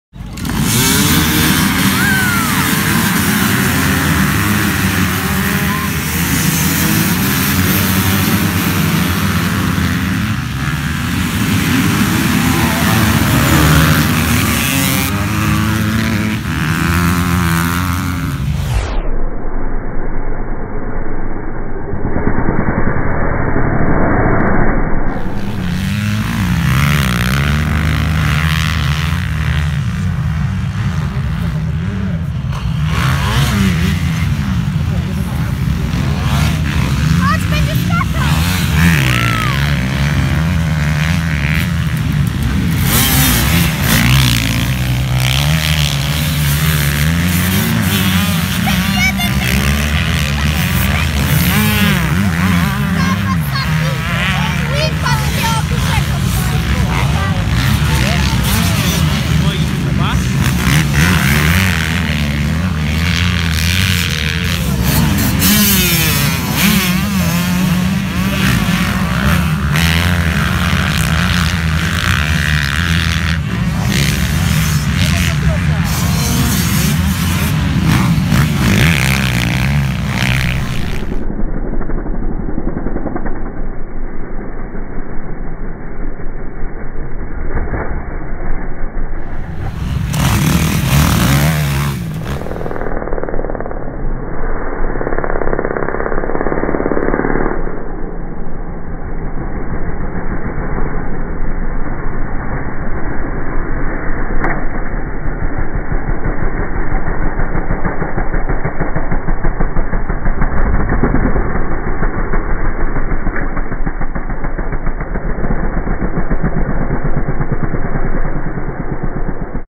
Seroki Cross Country 2019 (slow motion included)
Kilka ujęć z niedzielnego cross country w Serokach - start licencji oraz ujęcia w zwolnionym tempie.